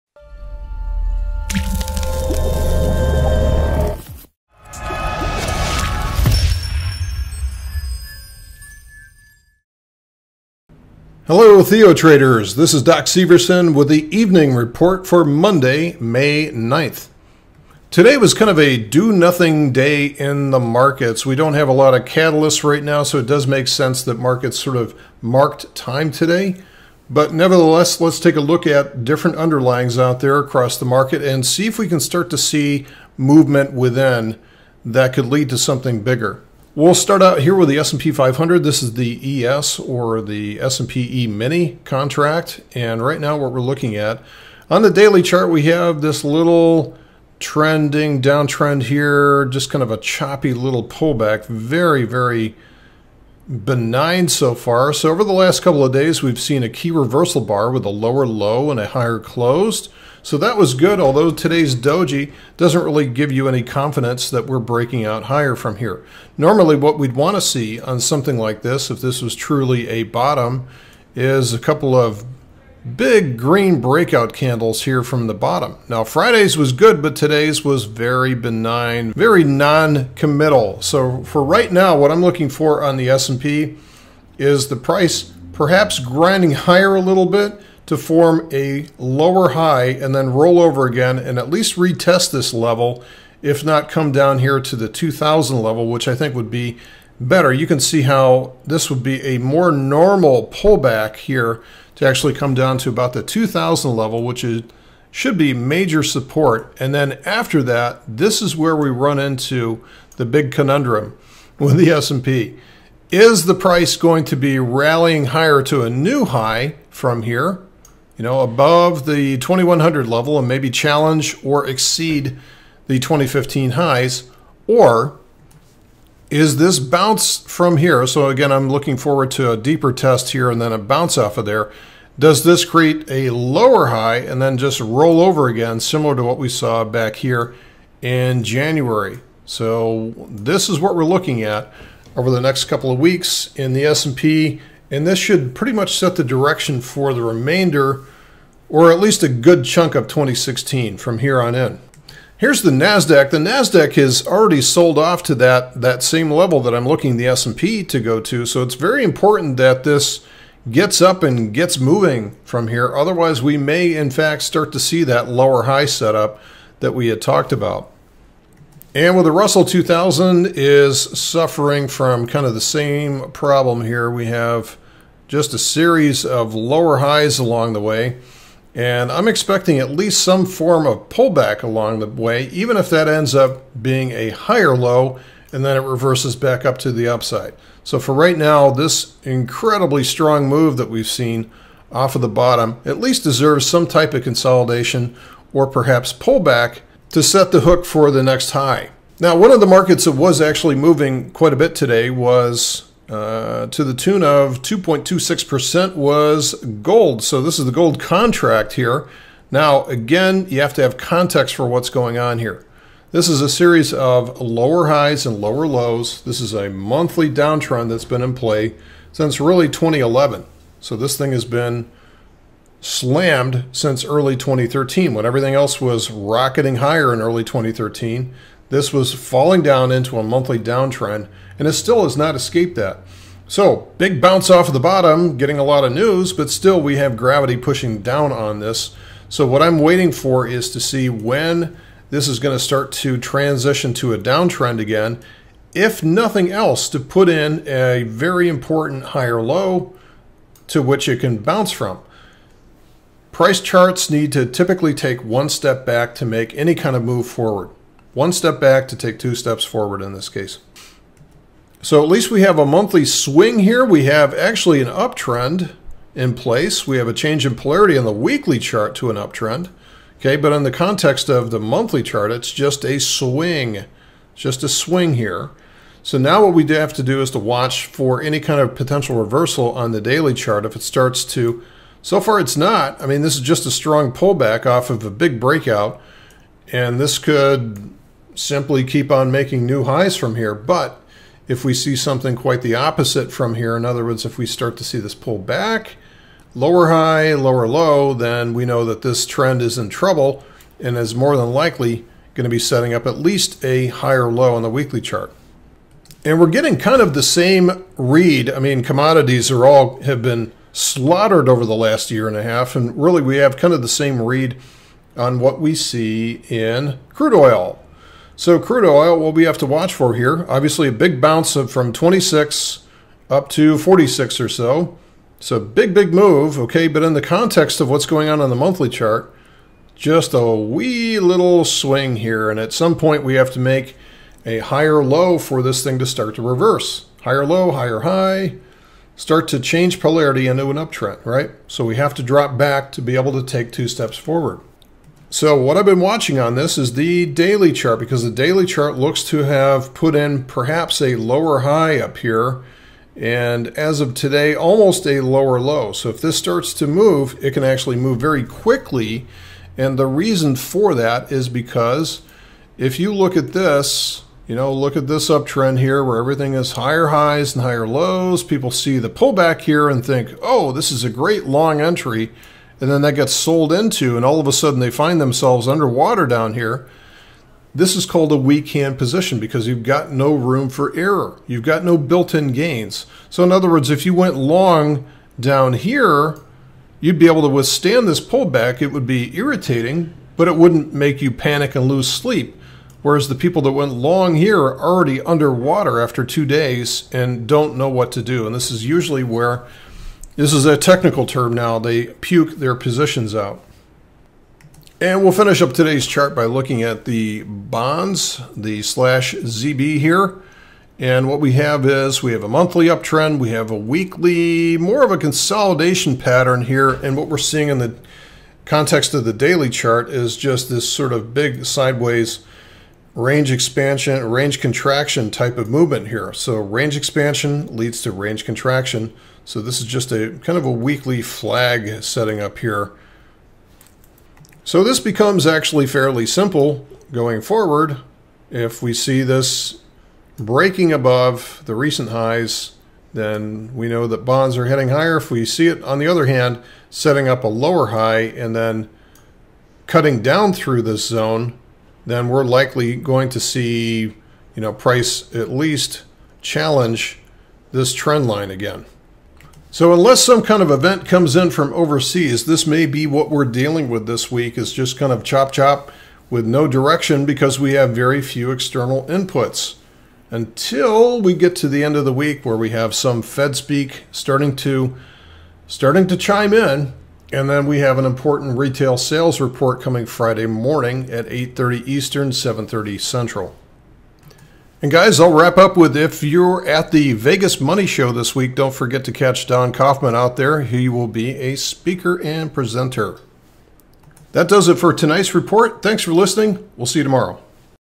Markets Search for Next Catalyst
On the surface it looked like a quiet day for stocks. However, other markets like gold and oil were selling off. Is gold and oil trying to give us a heads up as to the stock markets next move? Let's look at all the key markets to see where they may go from here.